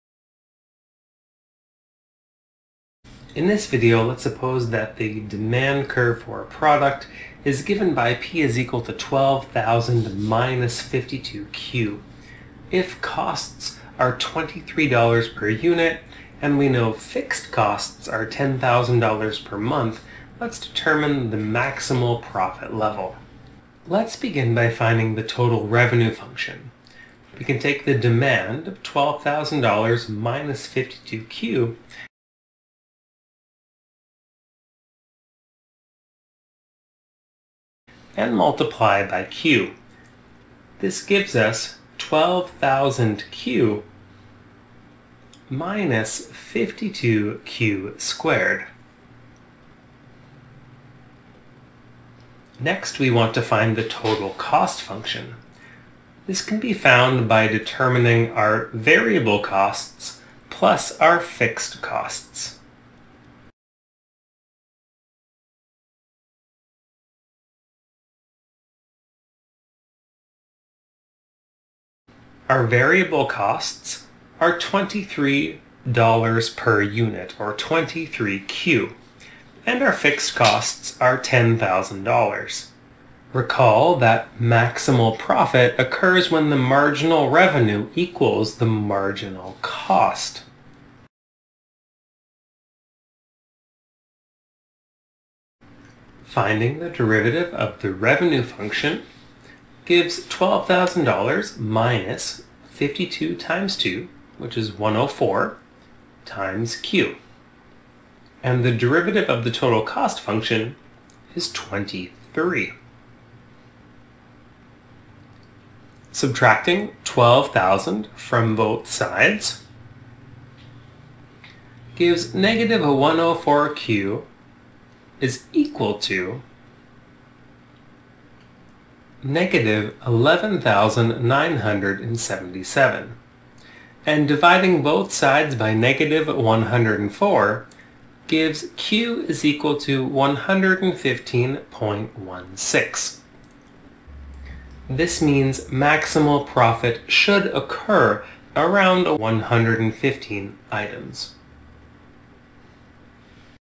Business Economics: Monopoly Q3
Learn how to solve a word problem involving maximal profit levels.

===LINKS===
FREE Business & Financial Mathematics Text: